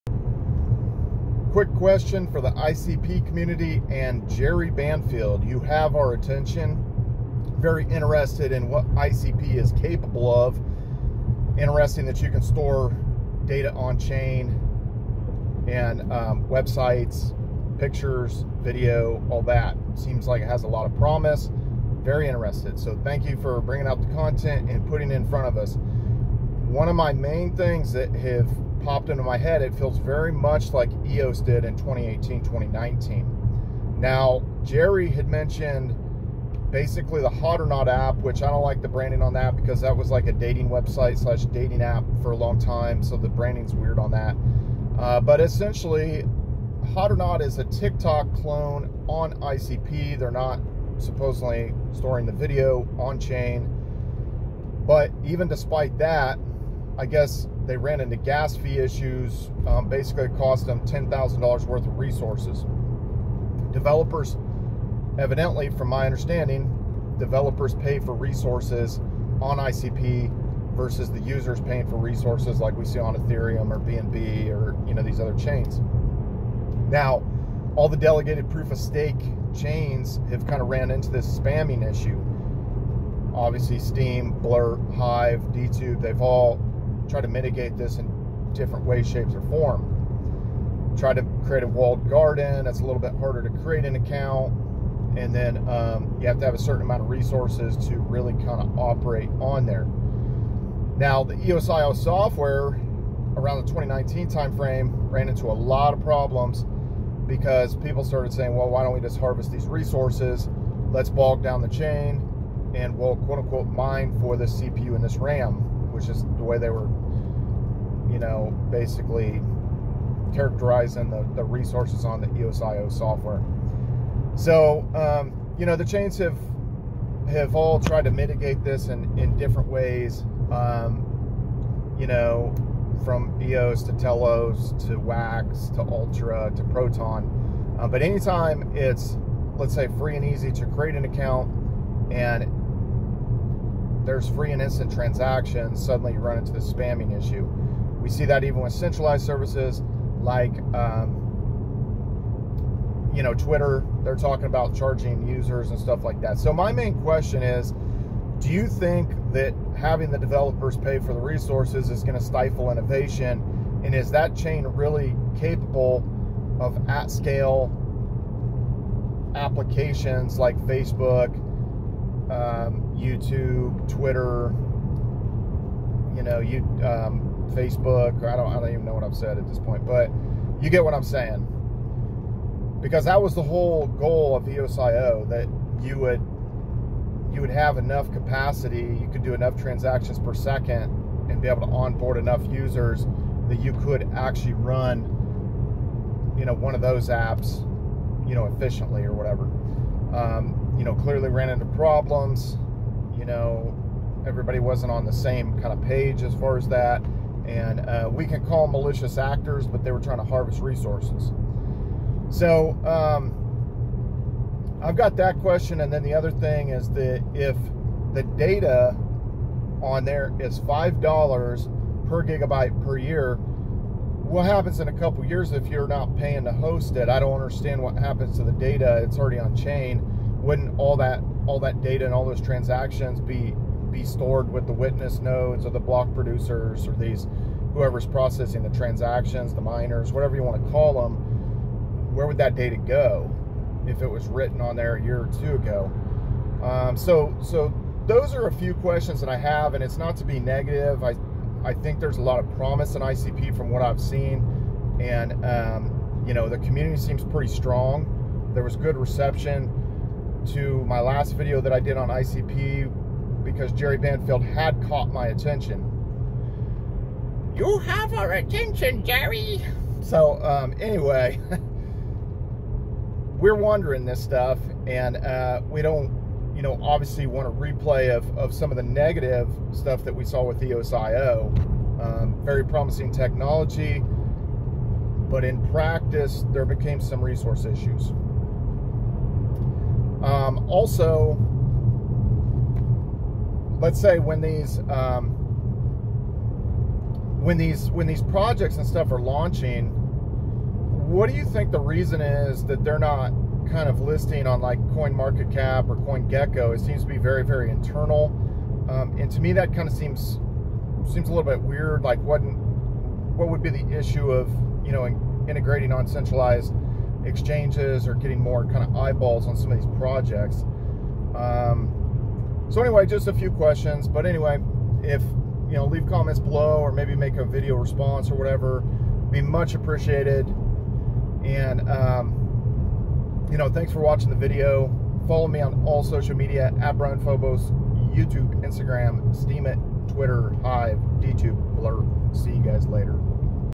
Internet Computer's Fatal Flaw? (Hard Questions For Jerry Banfield)
Jerry Banfield went balls deep into Internet Computer Protocol.  It caught my attention so I started watching a lot of his videos.  The one big thing that stuck out to me is it seems like developers pay the fees vs having users pay the fees.   All the Delegated Proof of Stake chains ran into this catch 22 where if everything is free then the spammers come in droves hitting the chain with thousands and thousands of spam messages and bog the system down.   They can also try to harvest resources from it like we saw with EOS where people were "mining" the cpu and ram resources.   It seems like a similar thing happened to a TikTok clone on ICP where suddenly it cost the developers a quick $10,000 because so many users got on the application.
My main questions for the community and Jerry Banfield is if they think this will slow down developers trying to build if it becomes cost prohibitive to on board significant amount of users.   Also if it is $5 / GB / Year to host pictures, video, data, etc on ICP what happens to the data if you don't keep paying?   Wouldn't it already be written to the blockchain?   Why would it disappear?